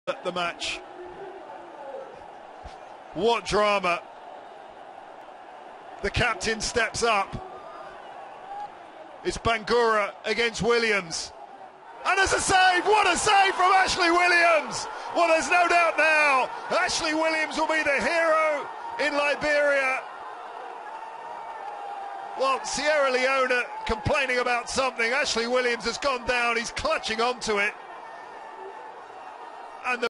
What a miss by Leone Star Captain you must watch
The penalty that make Leone Star fail to qualify for 2023 word cup